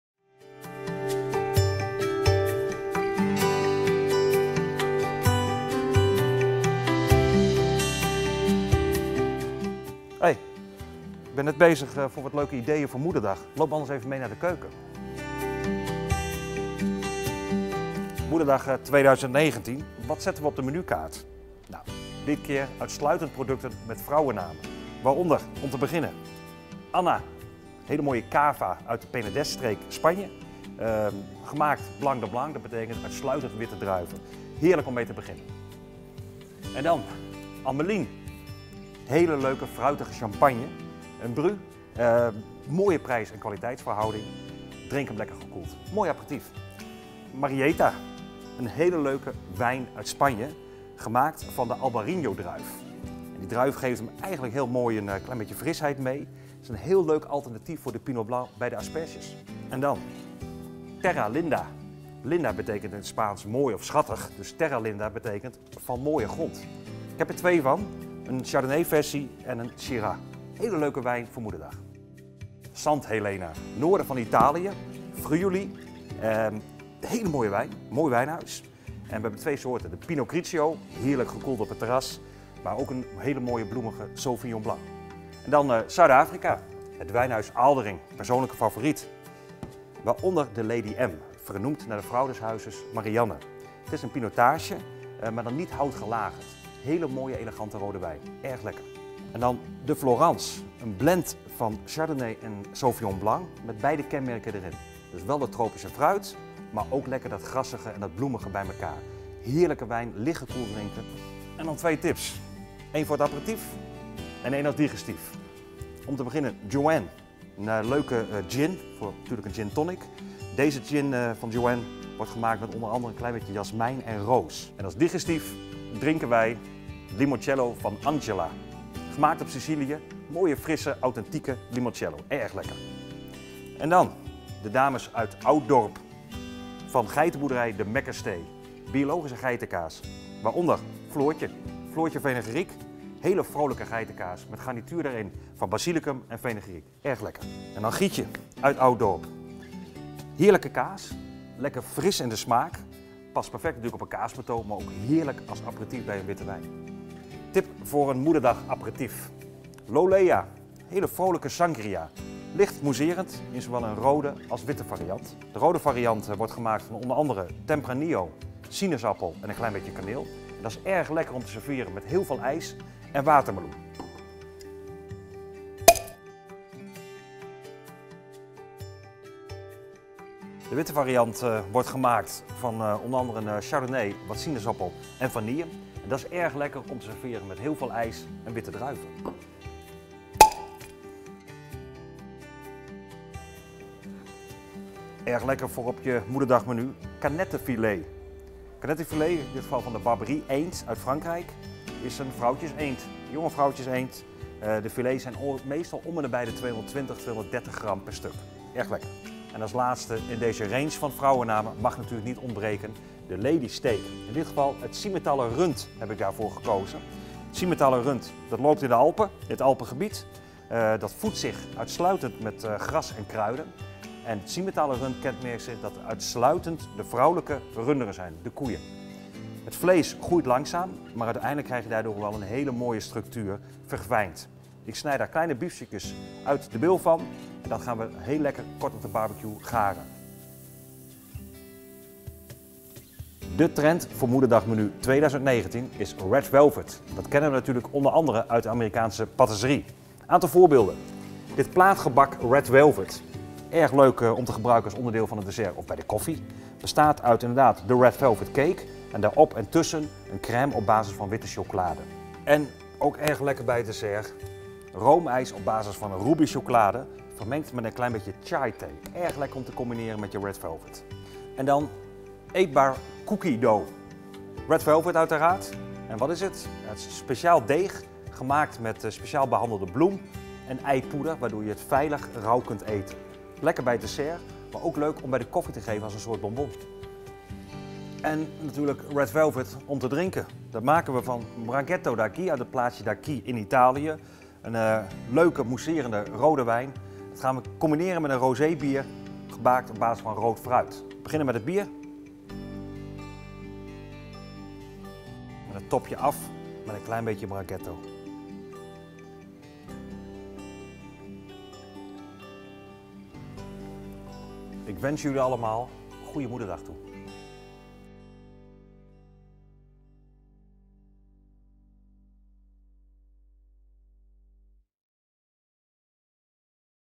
Moederdag tips
Moederdag 2019 komt eraan! Wat mag er niet ontbreken op de menukaart? Wij hebben een aantal goede suggesties voor je, met... vrouwennamen! Van 'Anna' cava tot 'Lolea' sangria, inclusief serveersuggesties. Daarnaast zoomen we in op de red velvet trend. Kijkt u mee?